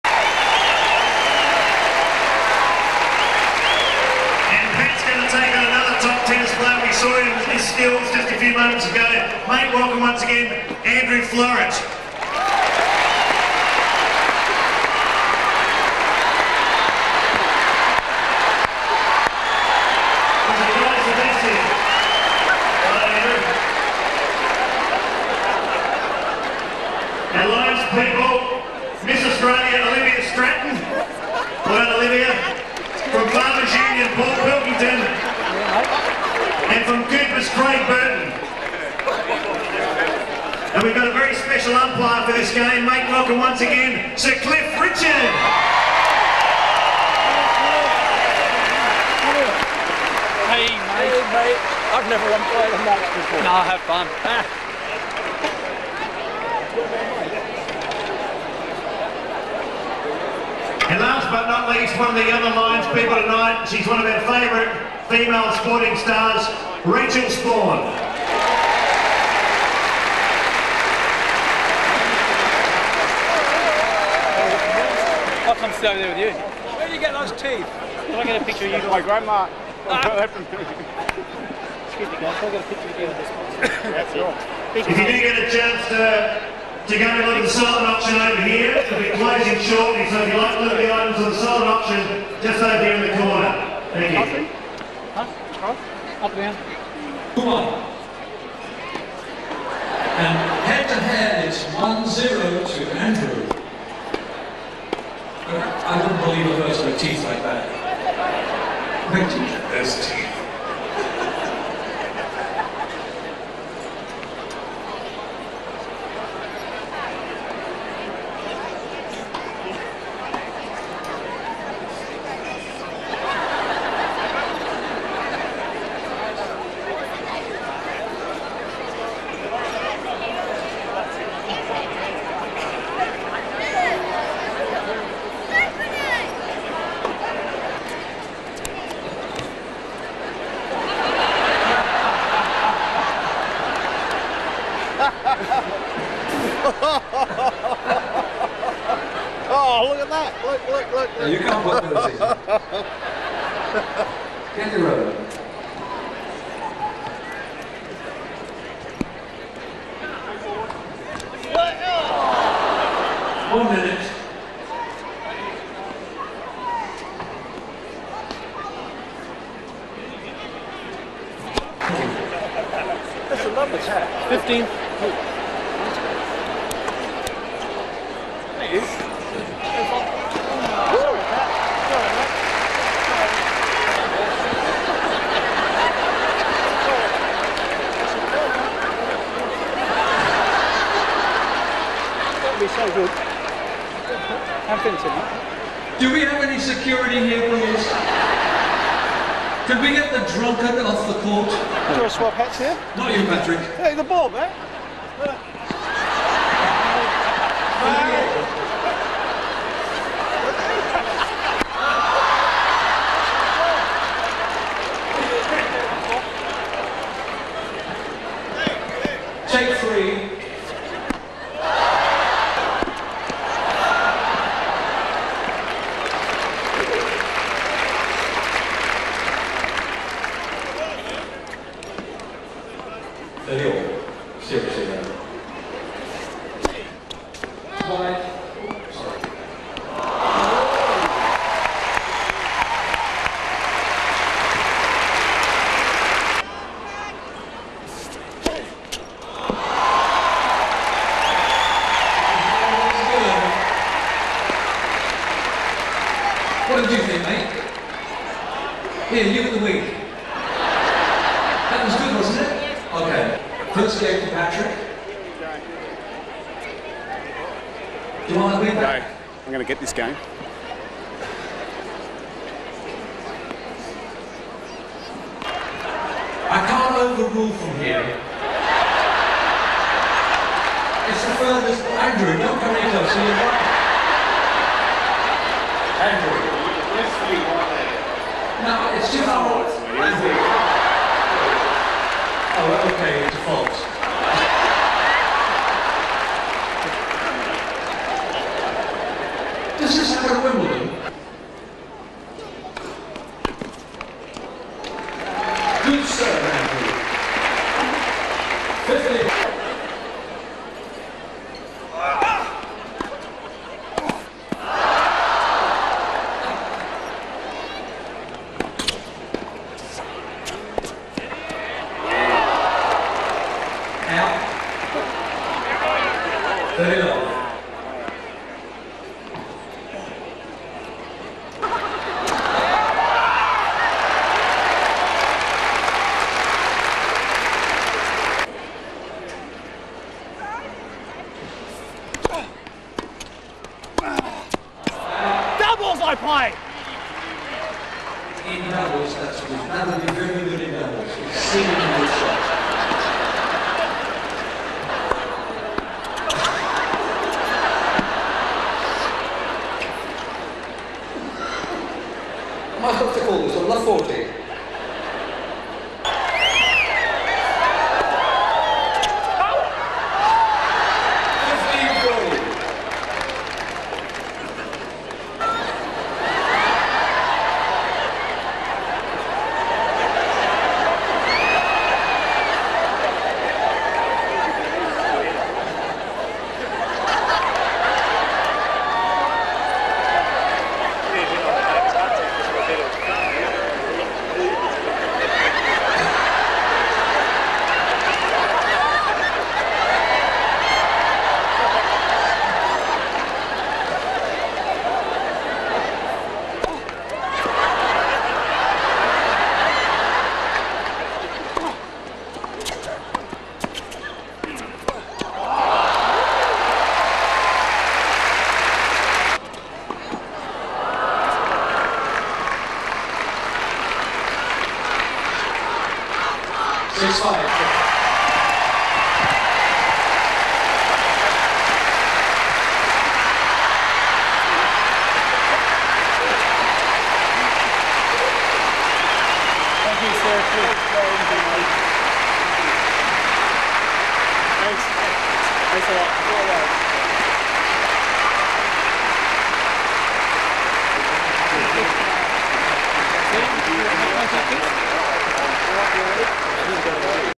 Cliff Richard at the Adelaide Celebrity Tennis Challenge
12 January 2004; at the Memorial Drive Centre Court Complex Cliff as umpire